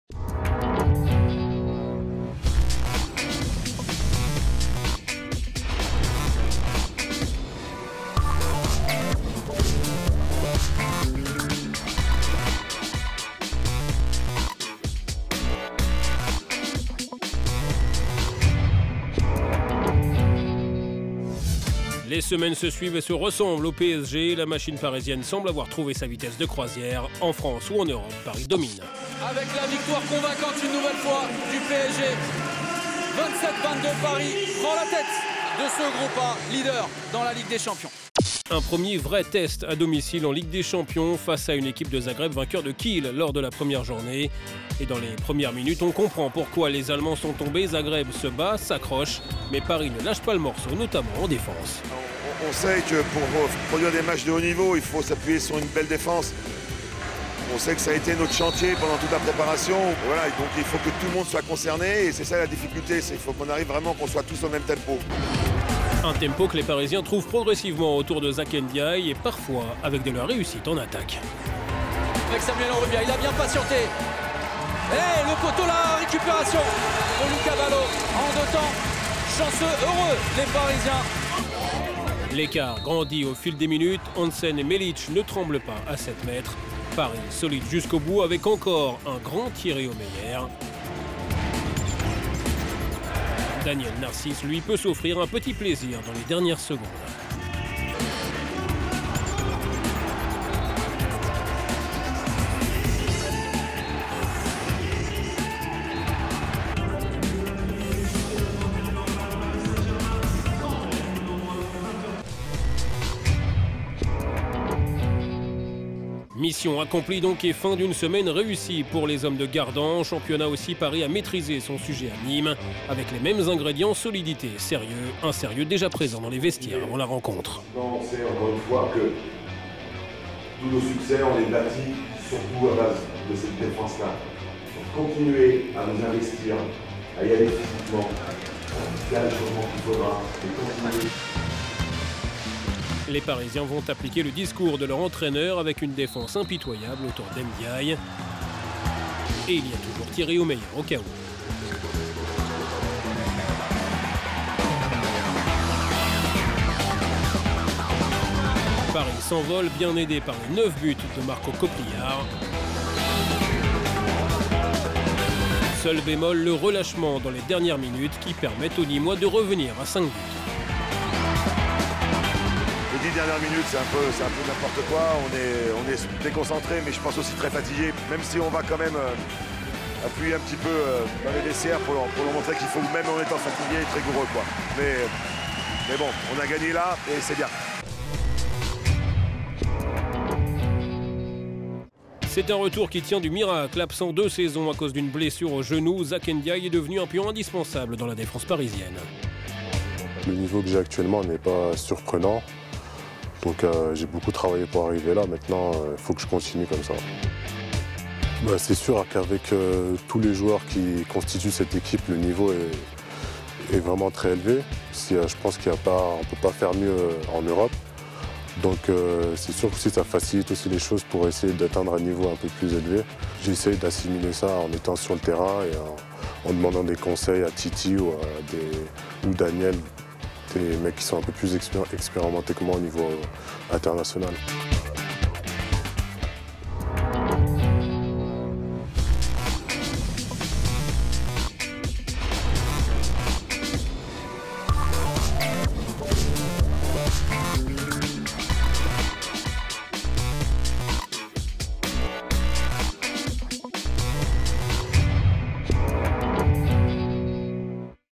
This is Paris : épisode 5
Au menu de ce nouvel épisode de This is Paris version handball :
-Retours sur les matchs face à Zagreb et Nîmes.
-L'interview de Zacharia N'Diaye qui signe son grand retour sur les terrains de handball